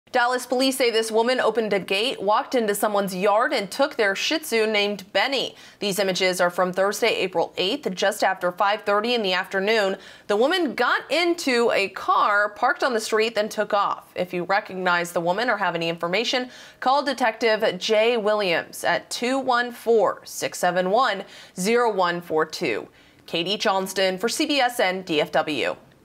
Woman Steals 'Benny' The Shih Tzu From Dallas Resident's Yard
Dallas Police are looking for a woman and the small dog she stole earlier this month. Katie Johnston reports.